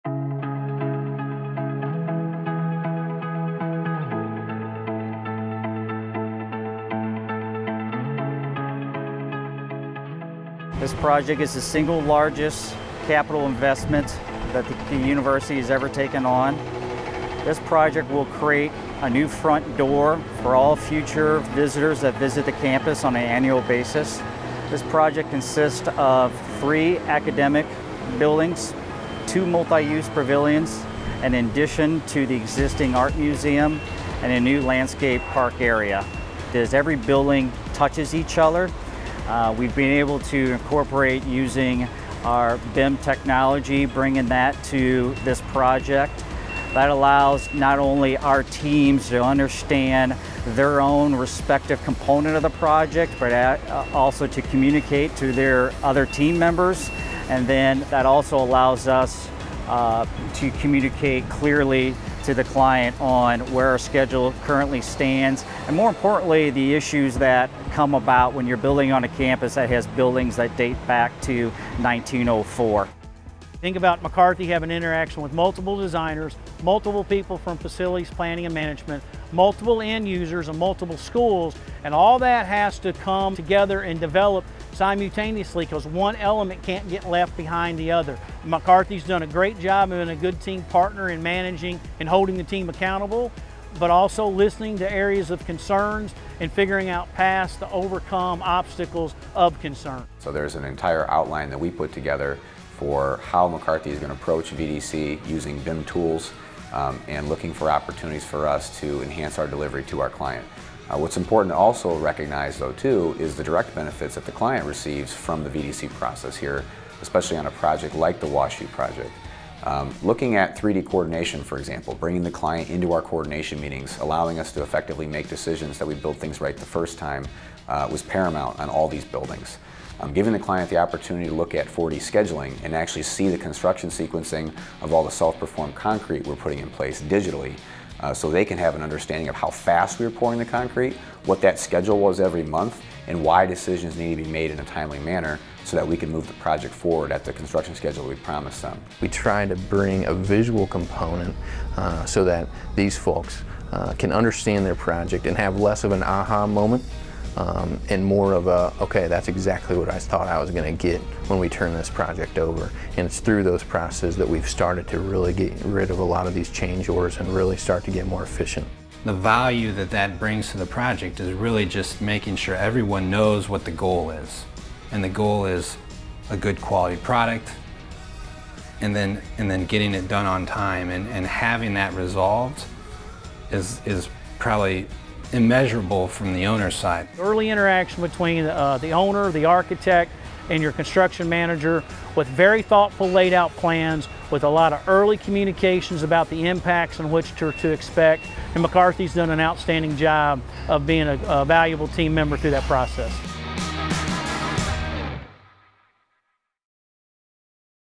McCarthy Transforms the East End of Washington University in St. Louis
Washington University in St. Louis teams with McCarthy to begin the largest capital project in the history of its Danforth Campus.